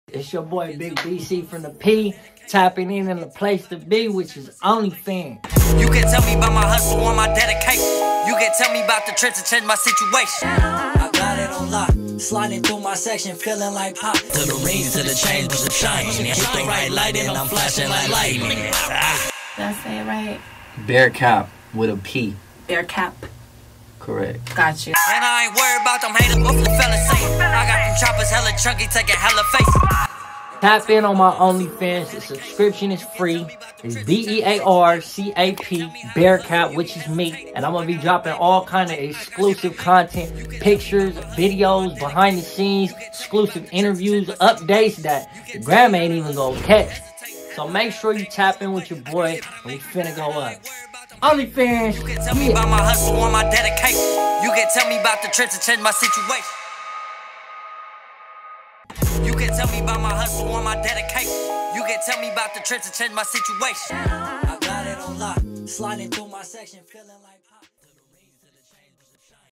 Bearcap | Rapper, Designer & OnlyFans Creator
Tap in on OnlyFans with ‘ya boy’ Bearcap! 🎙️💯

Rapper and designer Bearcap is here dropping all kinds of exclusive content and updates, from behind the scenes footage to unseen interviews and more! 🔊🔥